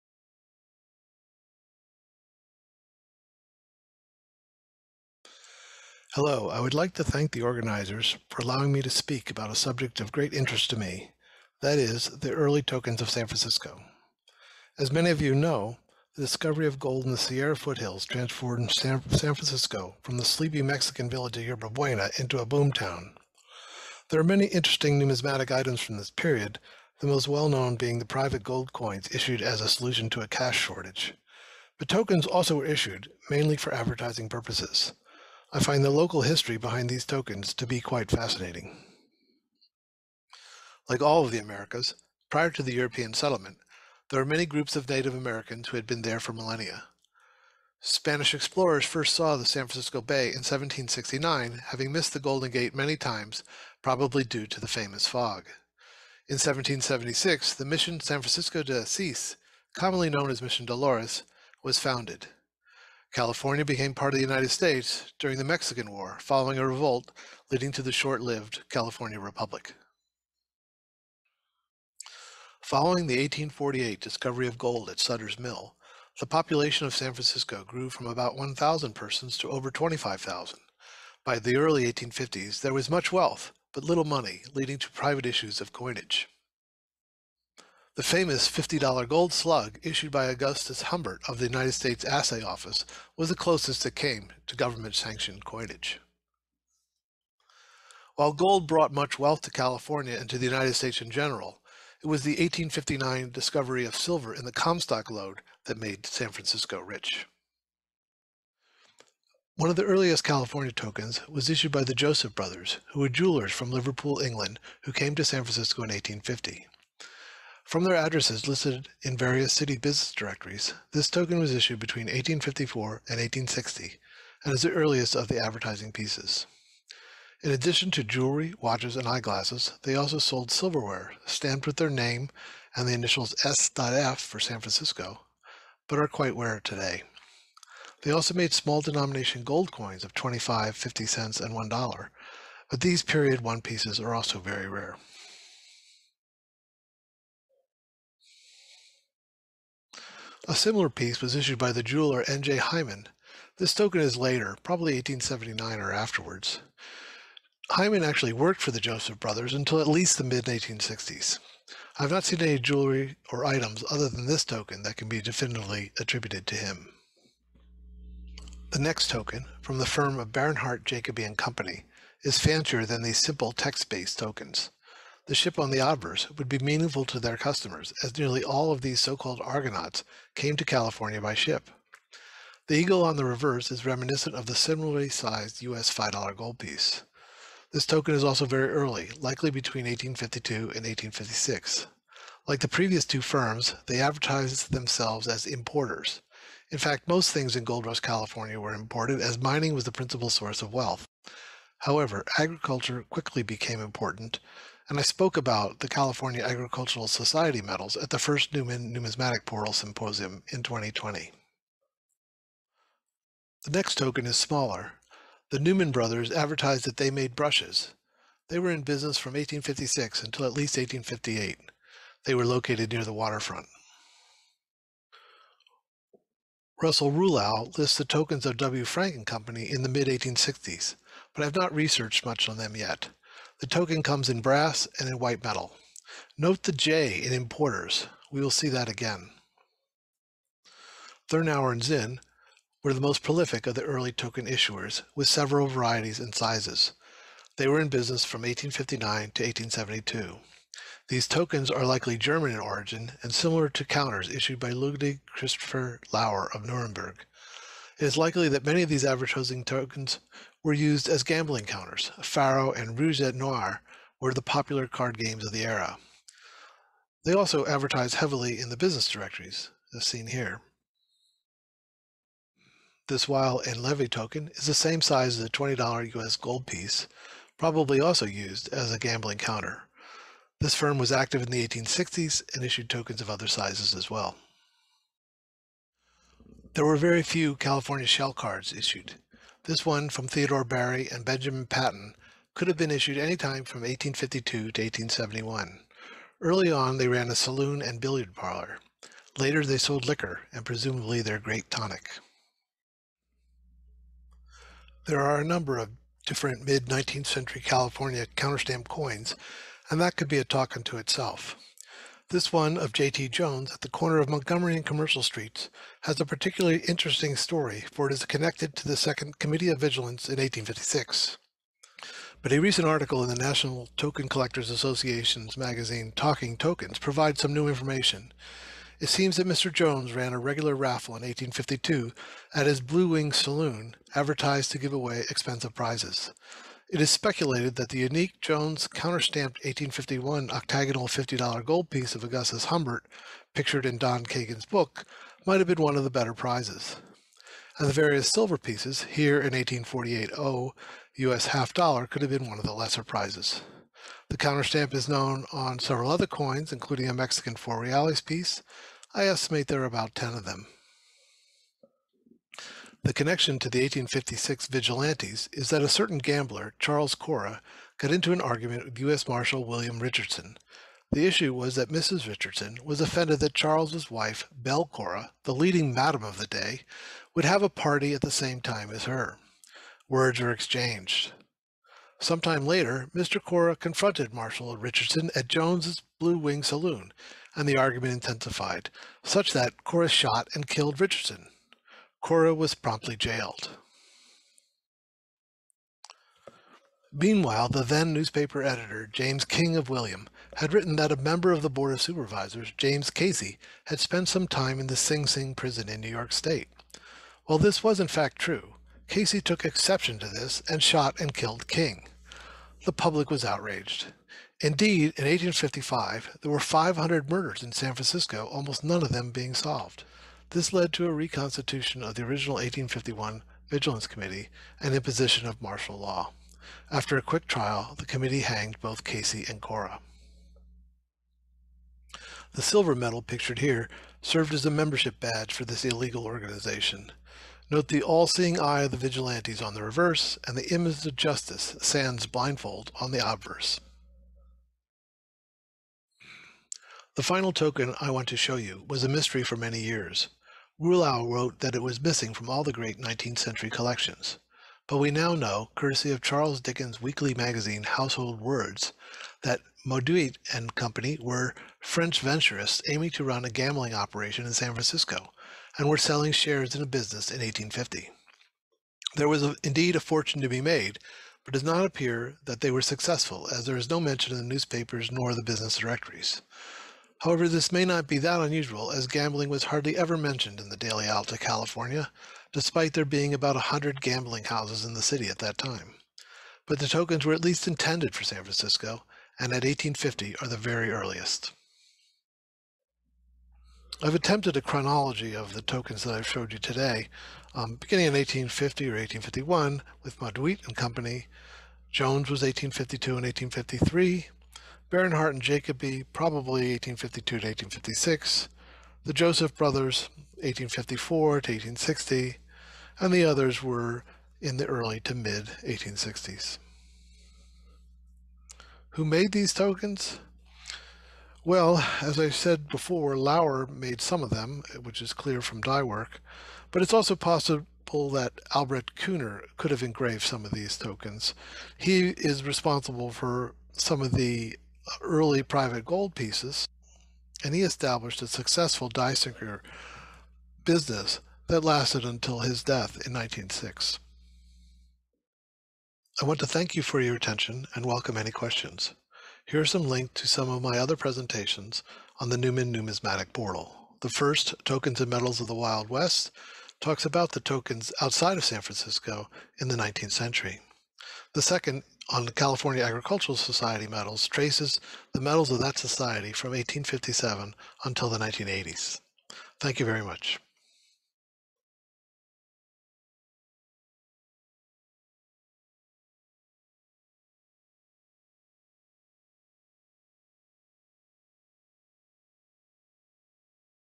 San Francisco, California Gold Rush Era Tokens - Michael Wehner
"San Francisco, California Gold Rush Era Tokens" - Michael Wehner (USA)
This lecture is part of the International Token Web Conference, 2021
(subtitles available, may be activated on the bottom left of the video)

The California Gold Rush of 1849 kick-started a westward migration in the United States. San Francisco went from a sleepy Spanish outpost to a bustling city in a manner of months. I will discus a selection of the earliest San Francisco merchant tokens beginning in 1849 to the early 1860s.